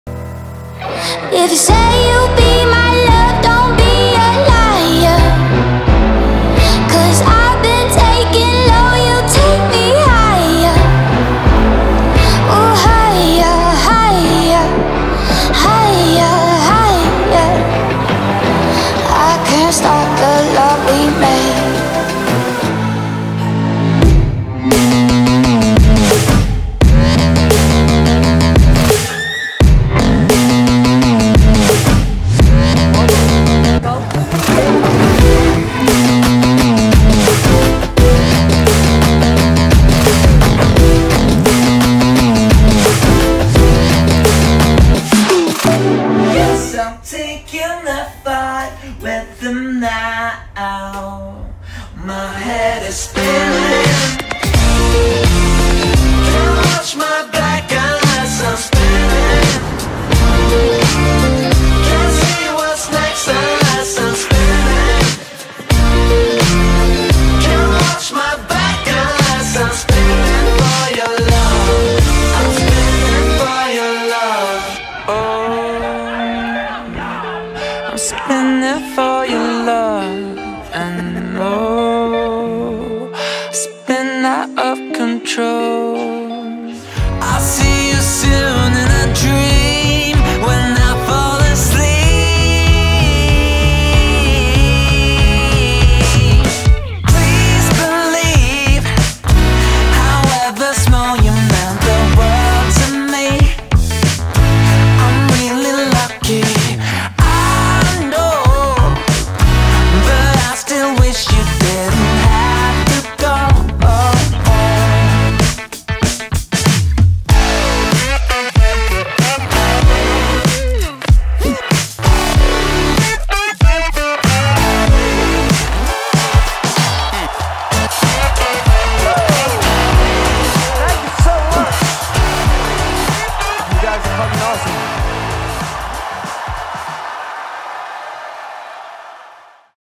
Summer recap 2018
Finally got around to get the full edit from the summer done lol. Touring festivals in the summertime is always amazing, we usually get to spend some free time the places we go unlike when we go on headline tours. Here’s footage from Portugal, Hungary, Netherlands and a couple spots in Norway. European headline tour with Dagny just finished, now we start our own Norway tour this week!